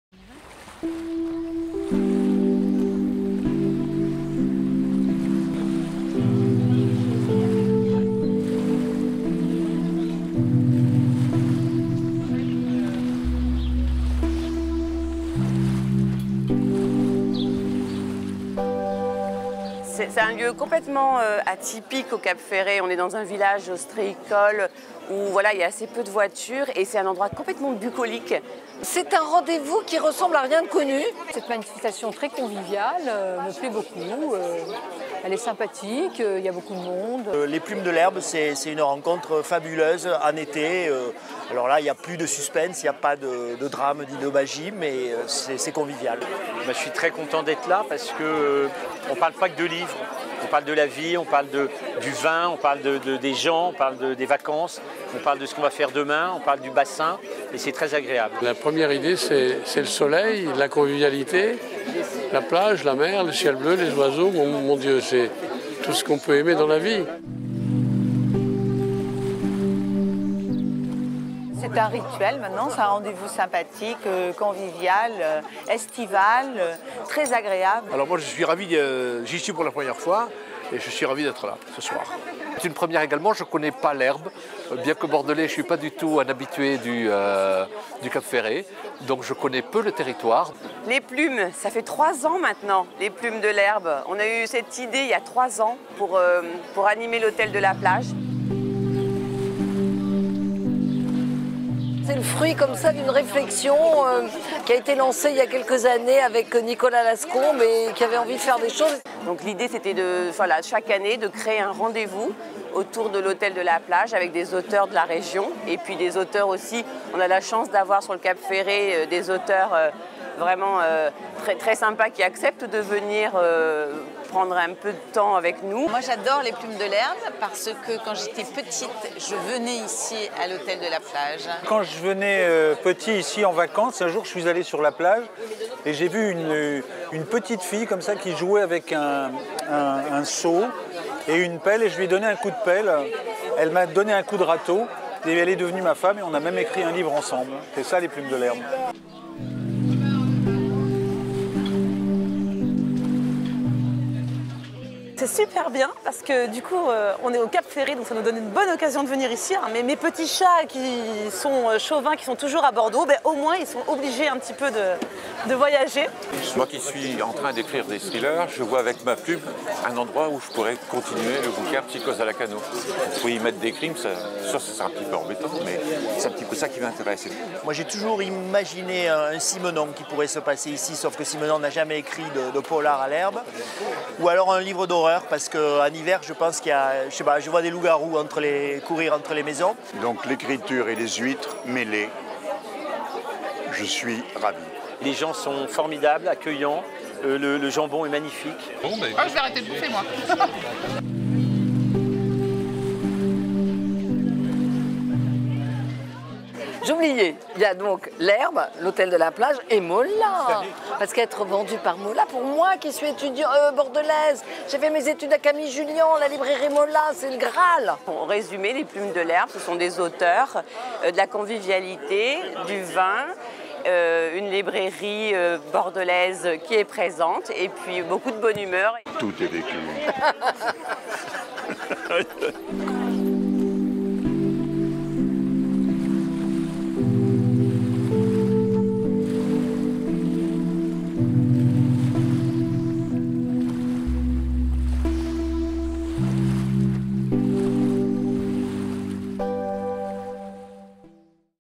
Les Plumes de l'Herbe - édition 2015
Retour en images sur l'édition 2015 des Plumes de l'Herbe : la rencontre avec une vingtaine d'auteurs et d'illustrateurs, au cœur de l'été, sur la terrasse de l'Hôtel de la Plage, Chez Magne, au Cap Ferret.